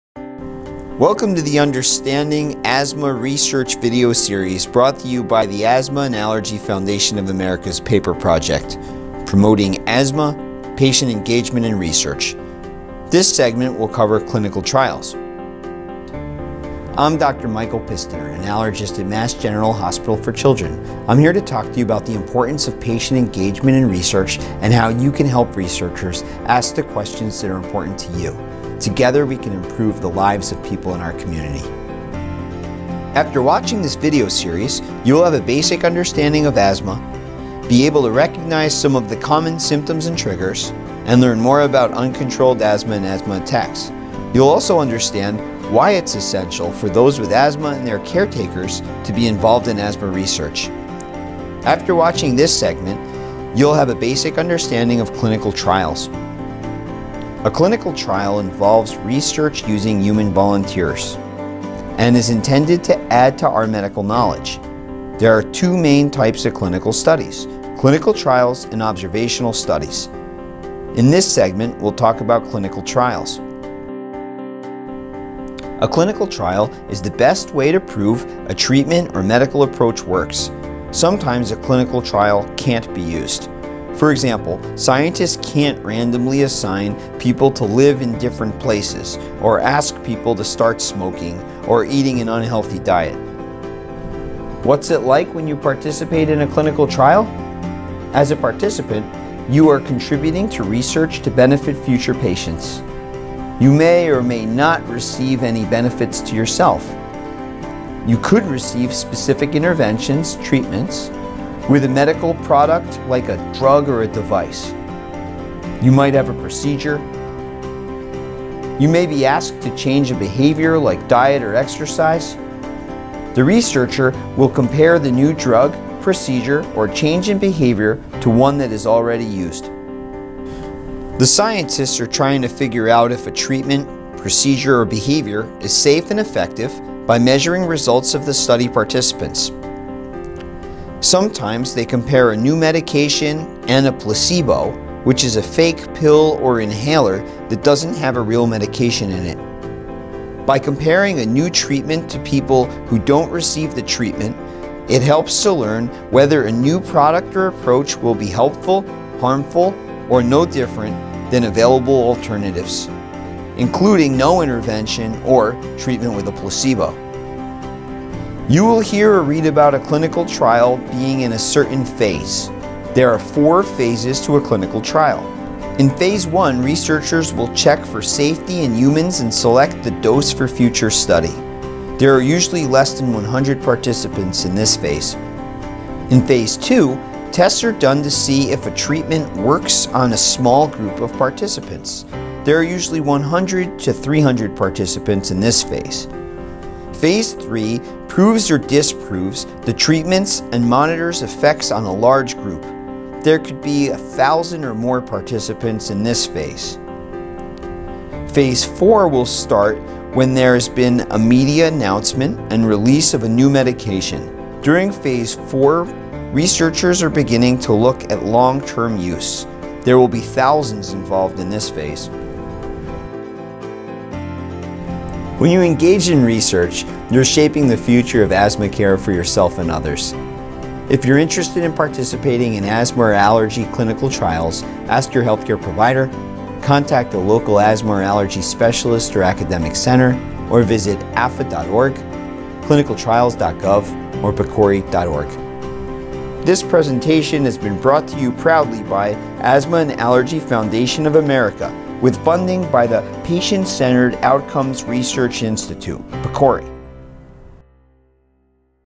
Understanding Clinical Trials
Many people participate in clinical trials to help researchers find new treatments. Find out more about the four phases of clinical trials and their role in asthma research. After watching this series, you will have a basic understanding of asthma and why it is important for patients to get involved in research. You can get involved in asthma research today by joining our online community and opting in to get alerts about research that might interest you